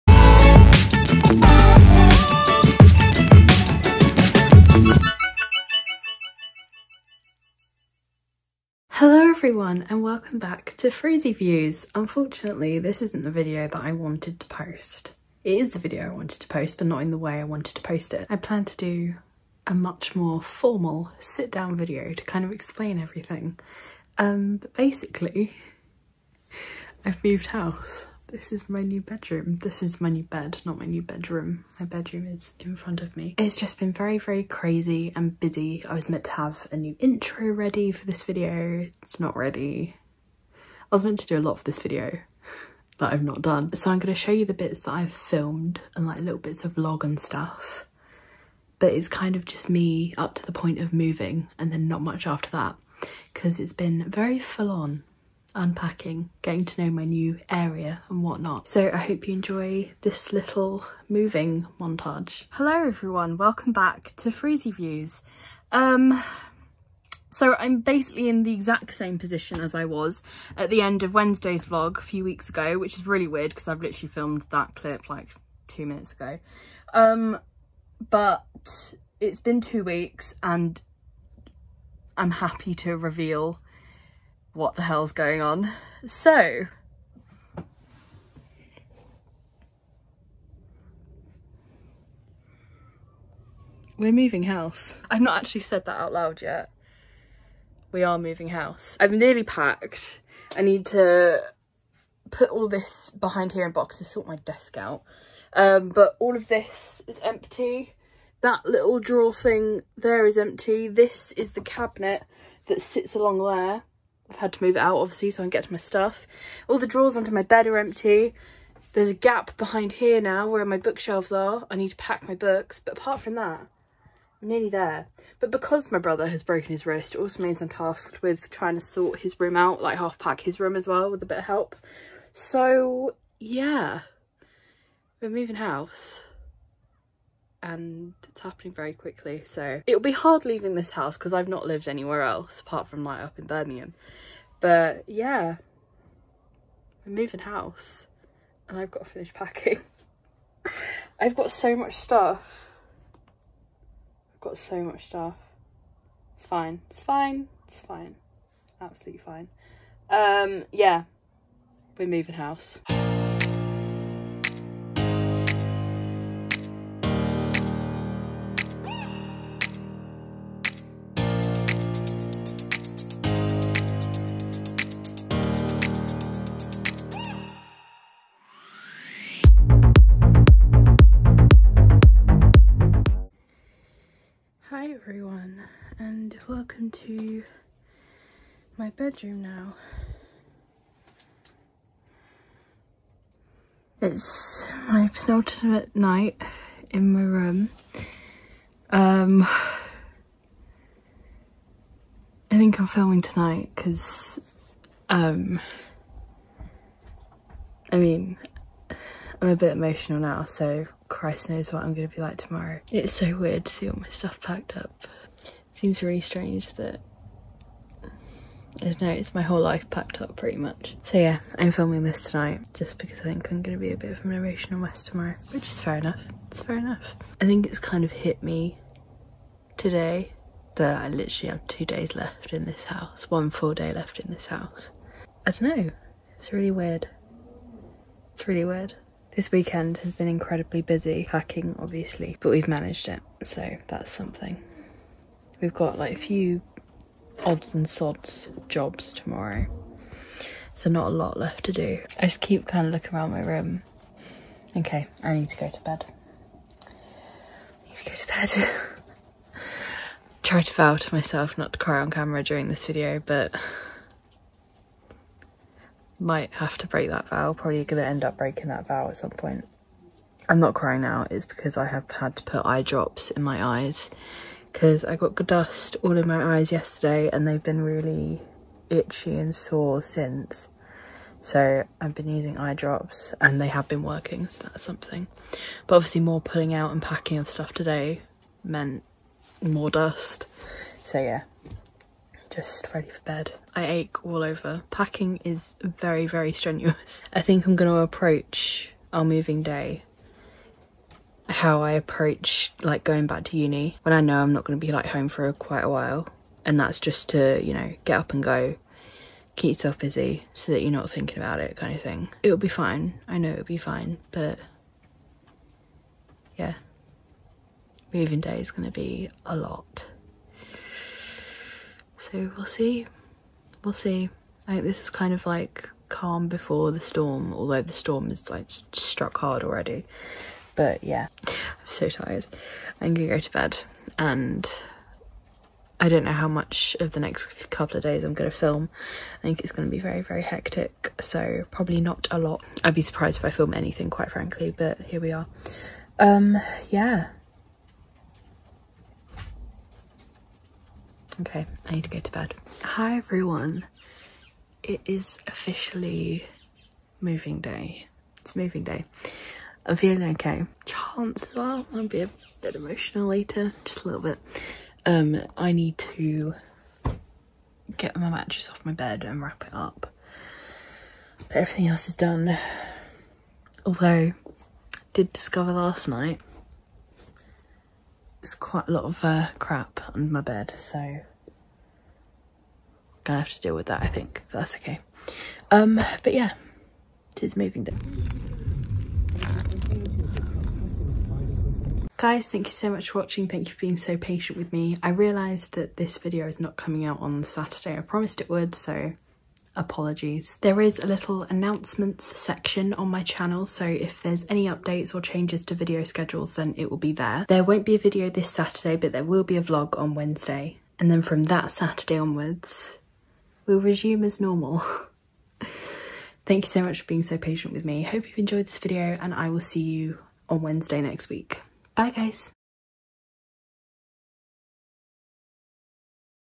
Moving House!
Hi Everyone!

Sorry that this video was a little later than expected, we've had wifi problems and only just been able to set it up. This isn't the video I wanted to post, the initial idea was a lot more formal, but I think you guys get the idea, we moved! Hope you enjoy the video and I'll see you next Wednesday!